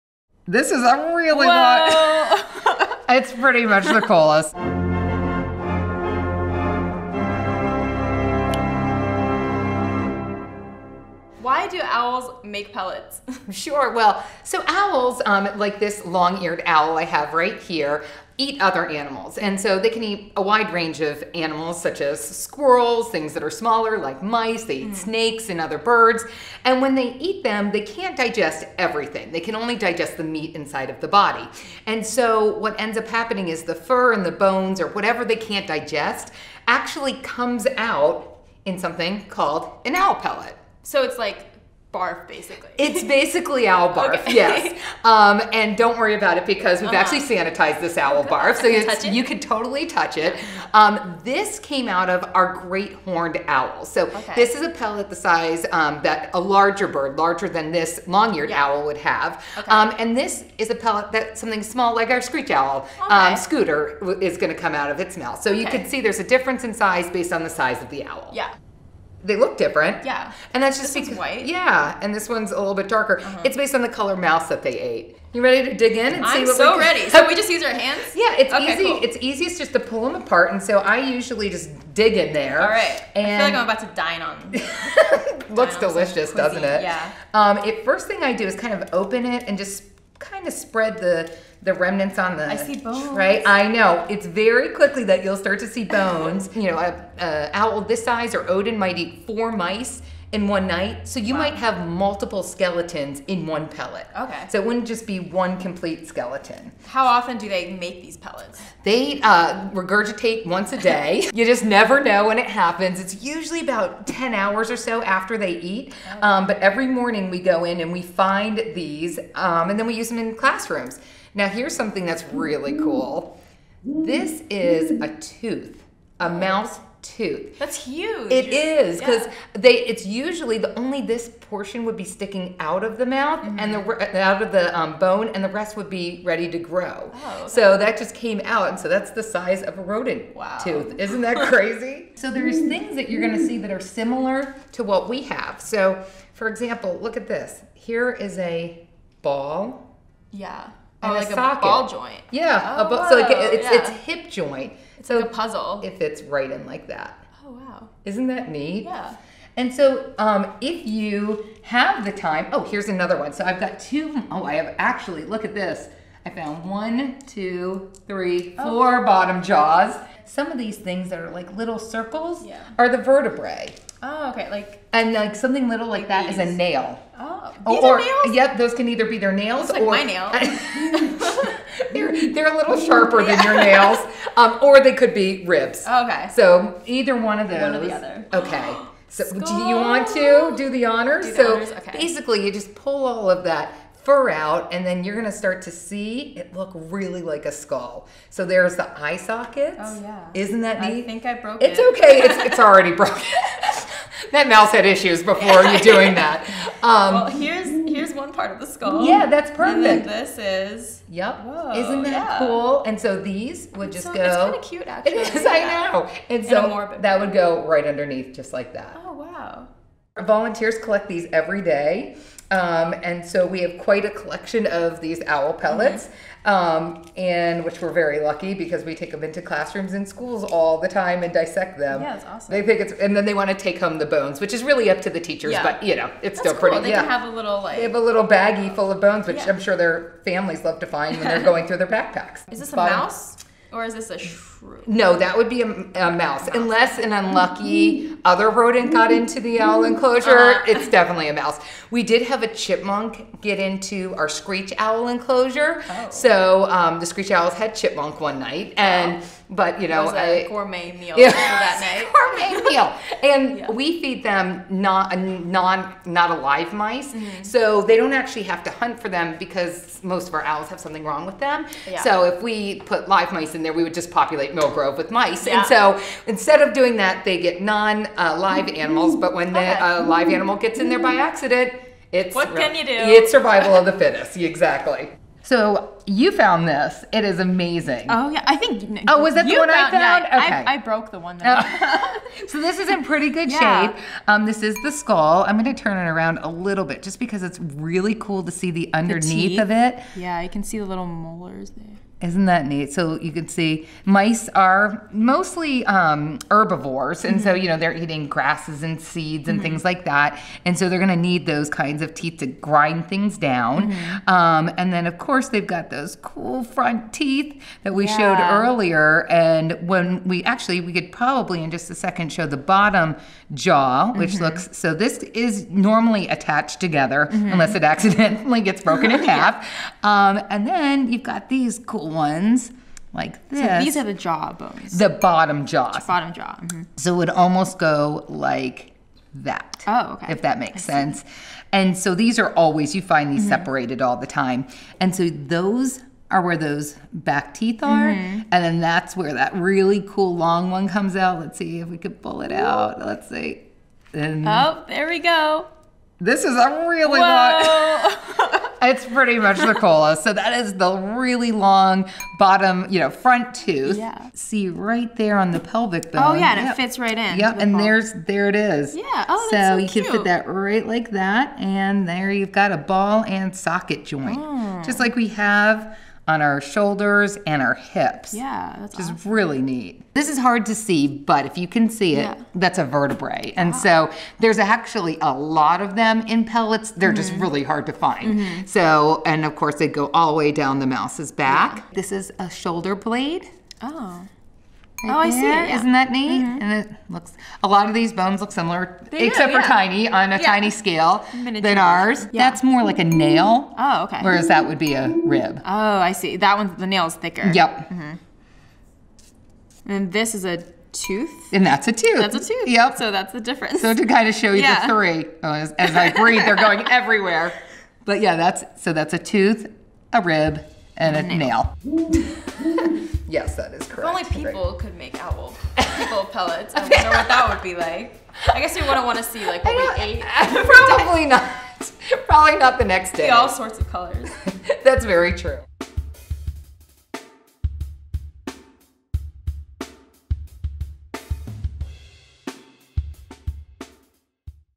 The Amazing Owls | Ep 2 | Pellet Dissection
Learn all about owls and their fascinating characteristics in our video series, The Amazing Owls! In the 2nd episode, we dissect owl pellets and learn about owls' eating habits.